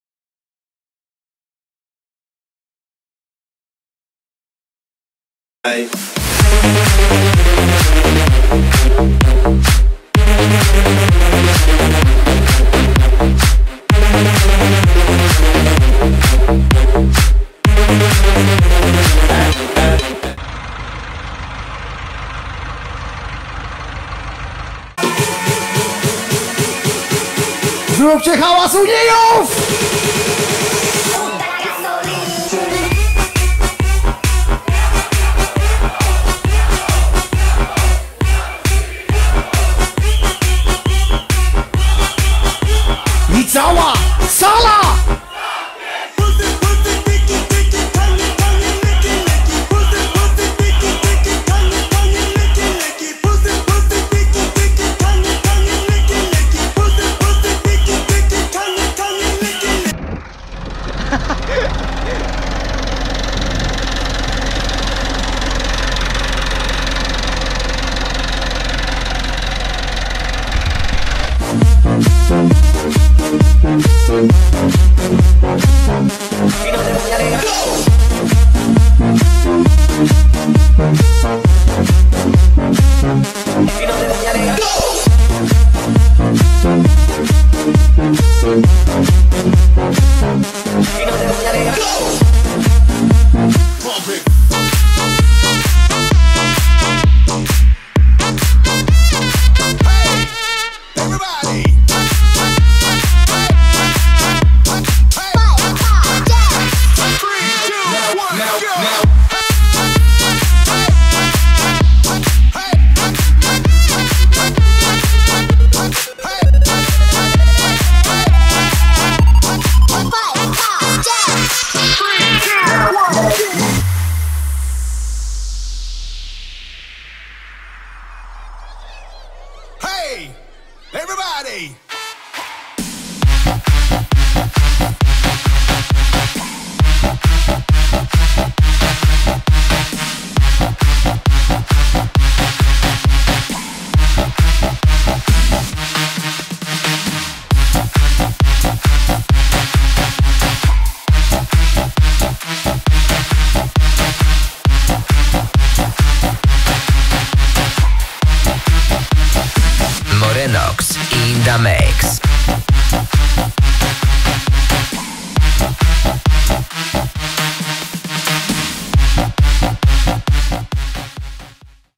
☆ [U-R-S-U-S bez KIEROWCY ?!] ☆ ✔(Kto jest szybszy (Oglądać do końca) POLECAM!!)
Witam Kłania się  Zakrzew TV

Podoba Ci się Zostaw suba łapę bo to ja lubię i kom :D dam serce :)

Wystąpili☛
URSUS 4512
Pług (podobny do Kvernelanda :D )